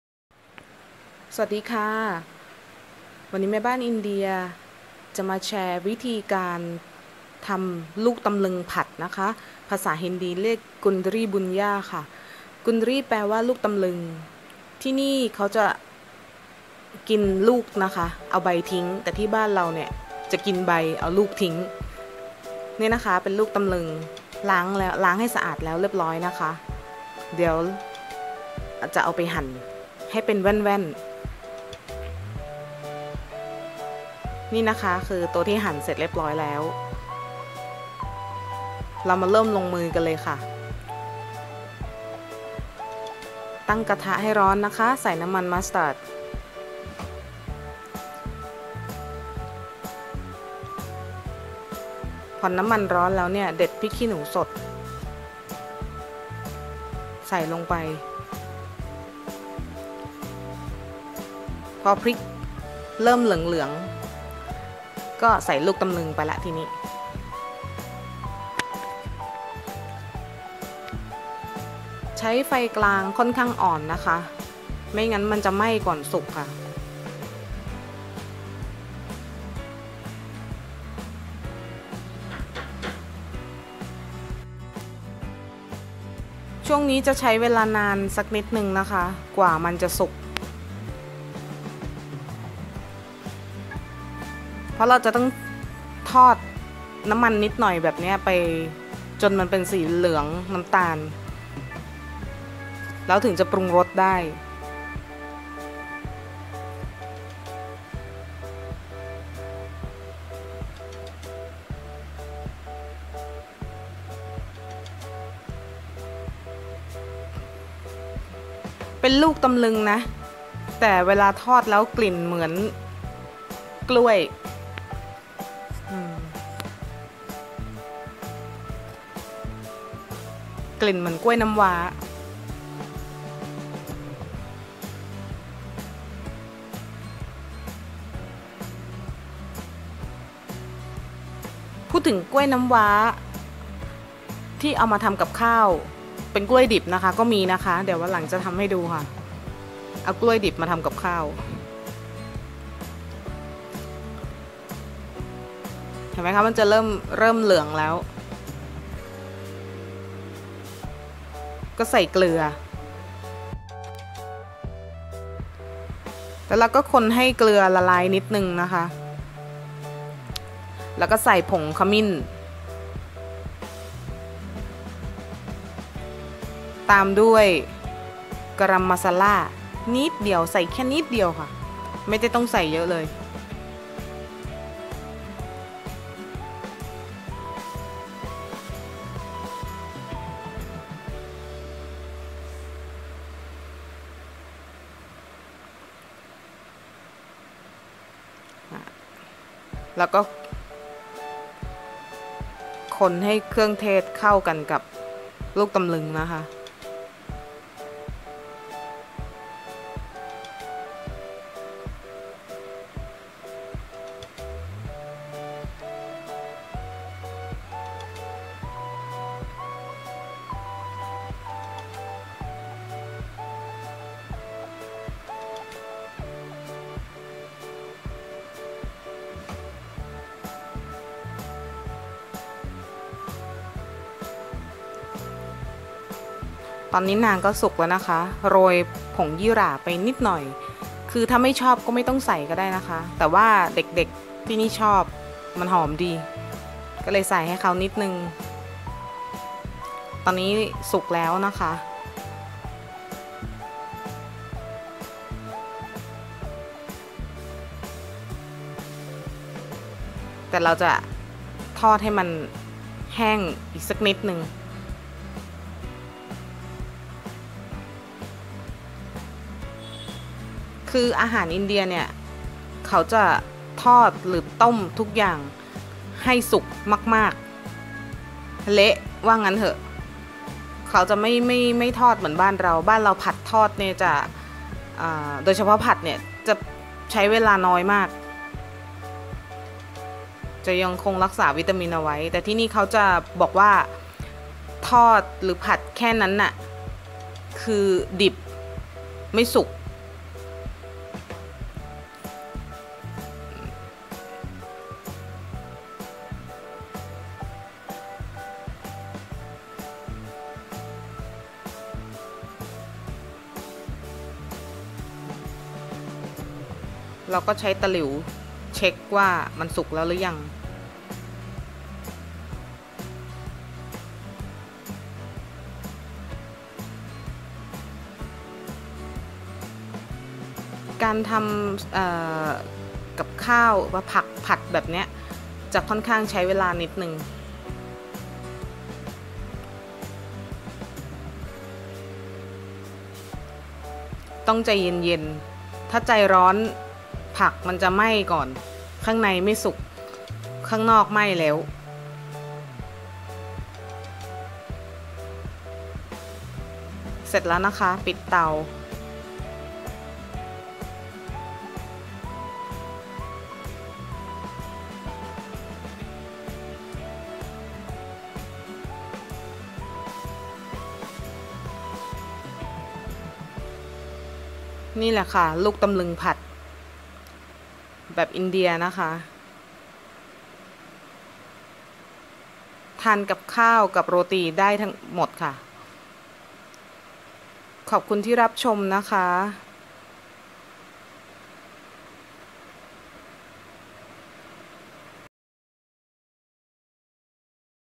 ลูกตำลึงผัด | Kundri Bhujia | อาหารซาคาฮารี EP.10
ลูกตำลึงหั่นแว่น น้ำมันมัสตาร์ด พริกขี้หนูสด เกลือ ผงขมิ้น การัมมาซาล่า ผงยี่หร่า